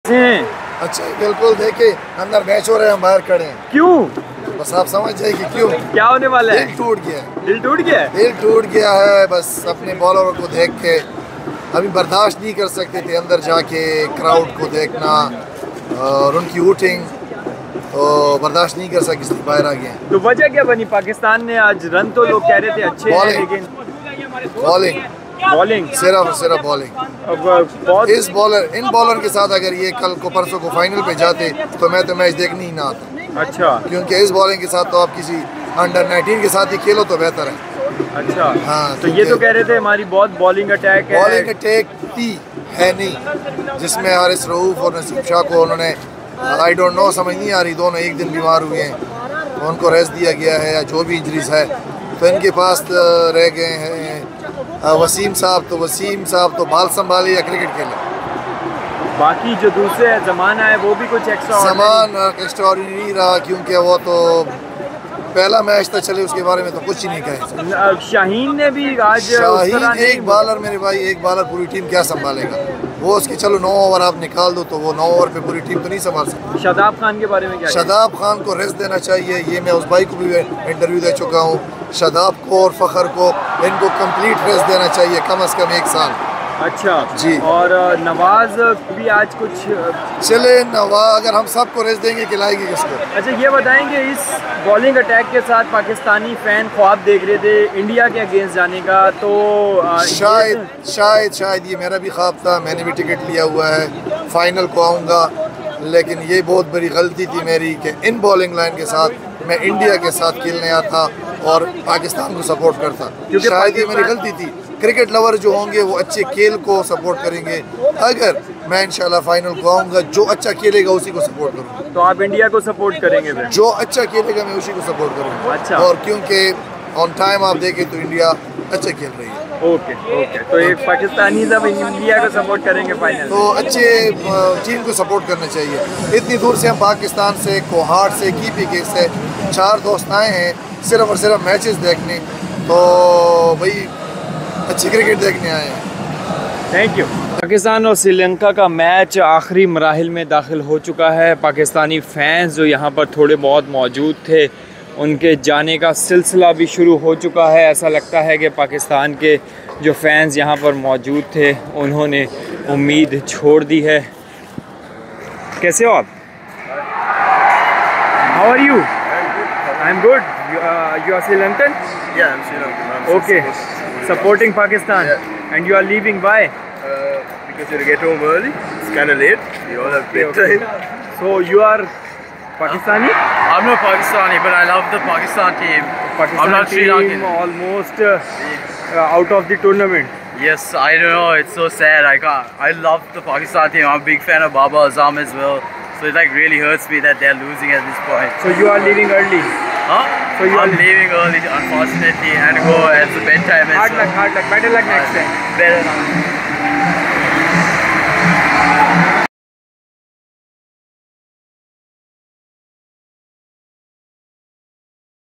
Heart breaks for Pakistani fans after loosing against Sri Lanka | PAK v SL | Asia Cup 2023 | Colombo
"Dil Toot Gaya, Is Bowling Line Ky Sath Hum Final Deserve Nahi Kartay", Pakistani fans disheartened.